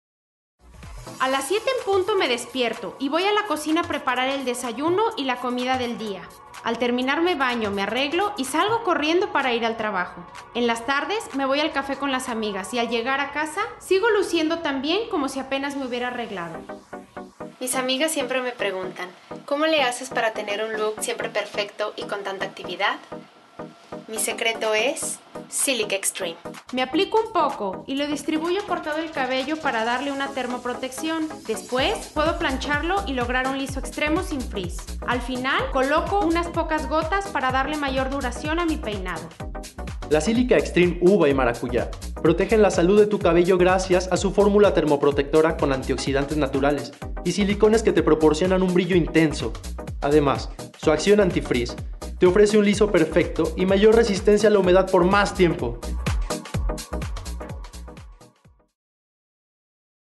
Silica Xtreme Unlimited
Las SILICAS XTREME UNLIMITED de MARACUYA y UVA posen una fórmula a base de antioxidantes y de silicones que proporcionan al cabello brillo intenso; gracias a su acción ANTI-FRIZZ se obtiene un lacio perfecto impidiendo que el cabello se esponje durante el día.